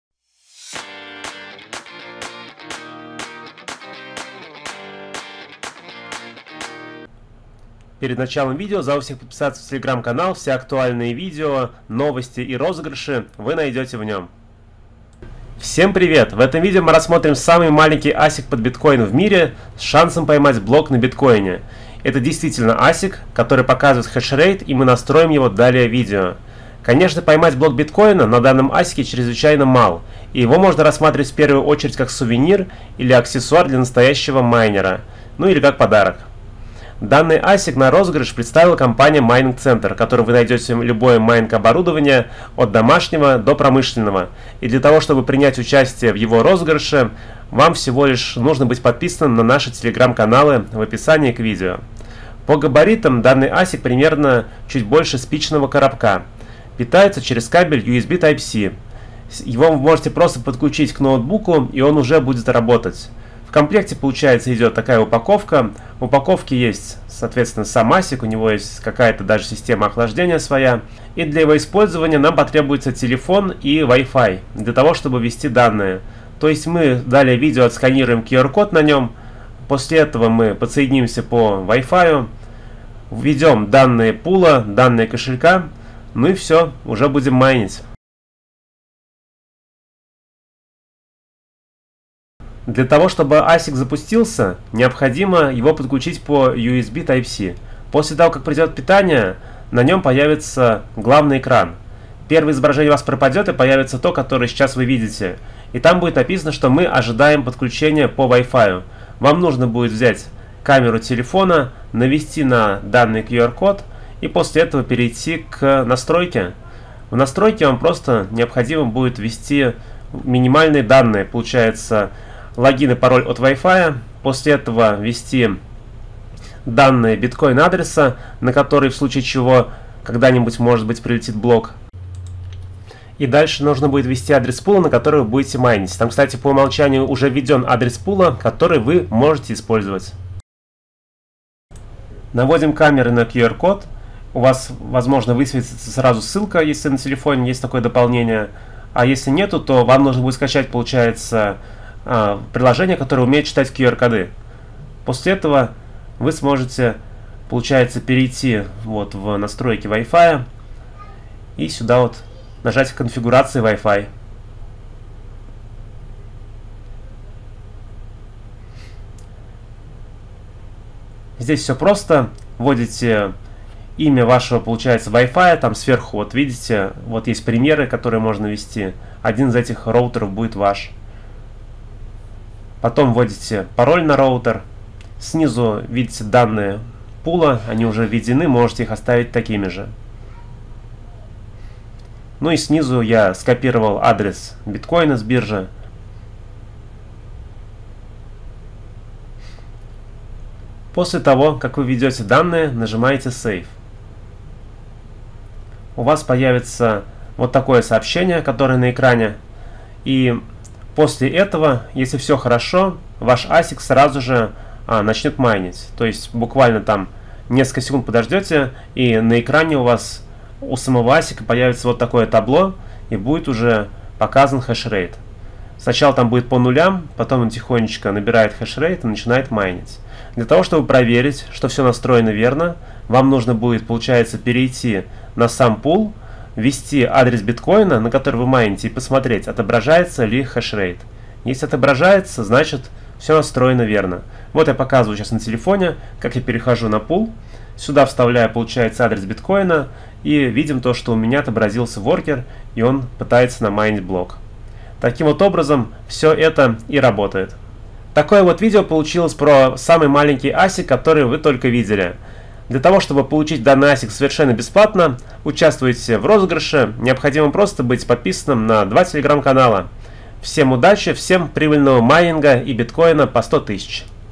🔥Самый маленький биткоин майнер в мире! Обзор и настройка Nerdminer и его розыгрыш!
🔥 Для участия в розыгрыше переходите в телеграмм бота